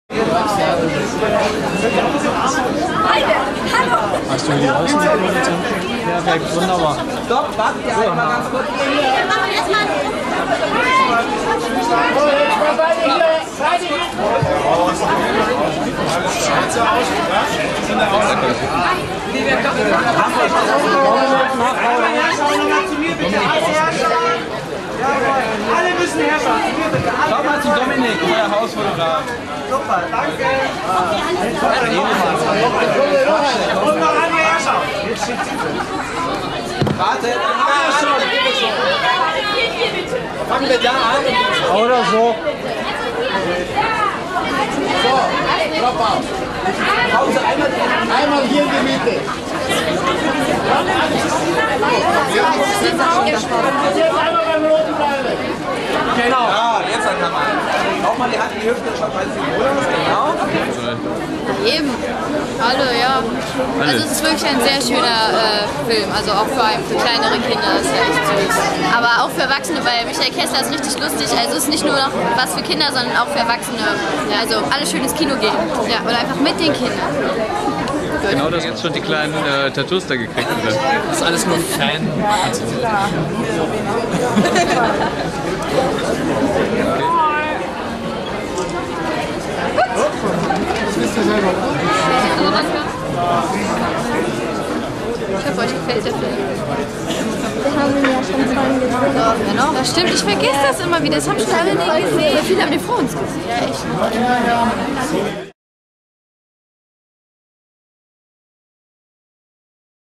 "Die Vampirschwestern" Filmpremiere im Mathäser Kino, München
"Die Vampirschwestern" Filmpremiere im Mathäser Kino, München unter anderem mit Laura Roge, die das Vampirmädel Dakaria spielt und Marta Martin als deren Schwester Silvania sowie Christiane Paul, die die menschliche Mutter der beiden spielt.